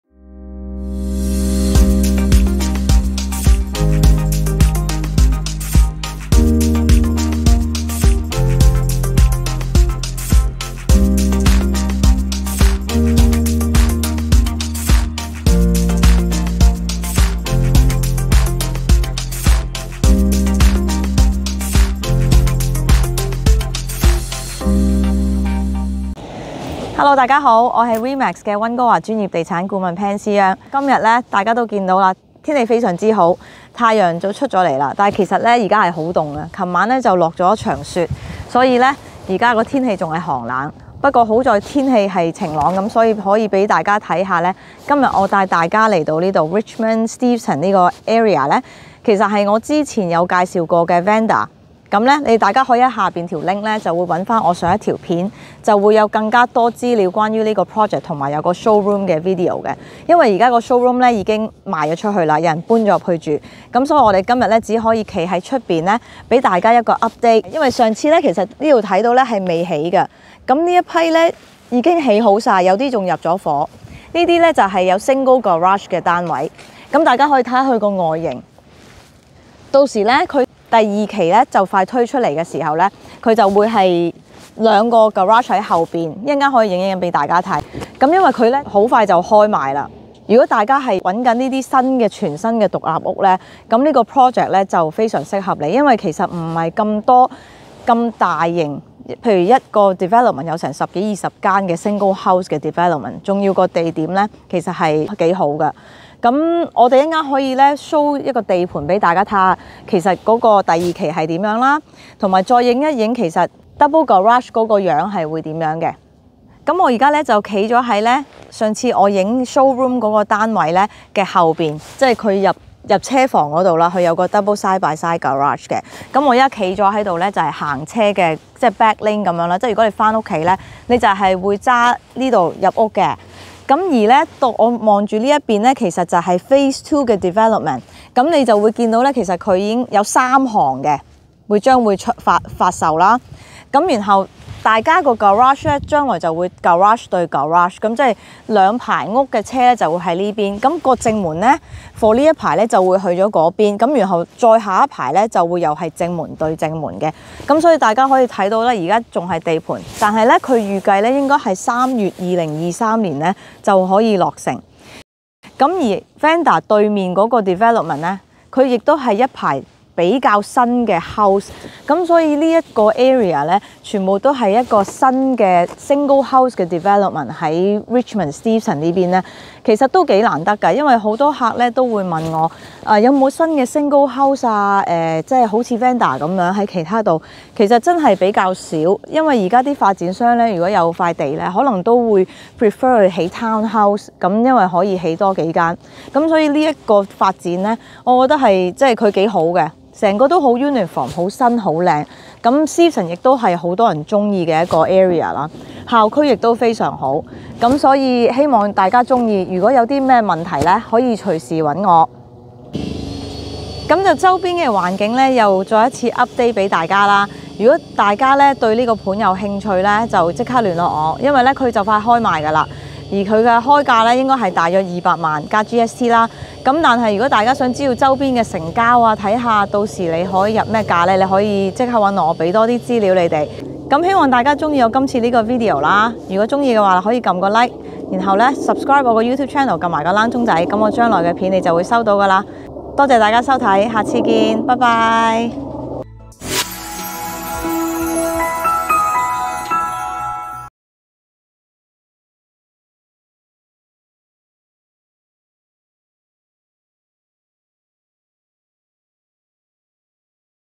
【溫哥華樓花】 Venda 第2期 更新/UPDATE | 列治文獨立屋樓花 | 期待已久 | Richmond Steveston | 加拿大温哥華專業地產顧問 | Pansy Yeung
Venda係一個新嘅獨立屋住宅開發項目。 該開發項目計劃於2023年三月完成。 面積由1881到1937方呎不等。

Venda is a new single family home development by iFortune currently under construction at Shinde Street, Richmond. The development is scheduled for completion in March 2023. Sizes range from 1881 to 1937 square feet.